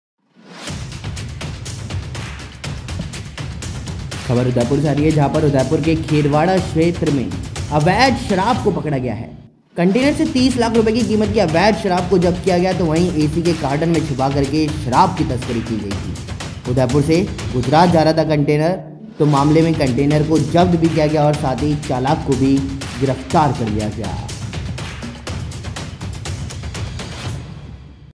उदयपुर के खेरवाड़ा में अवैध शराब पकड़ी Illegal liquor caught in Kherwara, Udaipur
उदयपुर के खेरवाड़ा में अवैध शराब पकड़ी
कंटेनर से 30 लाख रुपए कीमत की शराब जब्त
एसी के कार्टन में छिपाकर शराब की तस्करी
उदयपुर से गुजरात जा रहा था कंटेनर
मामले में कंटेनर जब्त, चालक गिरफ्तार
Illegal liquor caught in Kherwara, Udaipur
Liquor worth Rs 30 lakh seized from container
Liquor smuggling by hiding in AC carton
The container was going from Udaipur to Gujarat
Container seized in case, driver arrested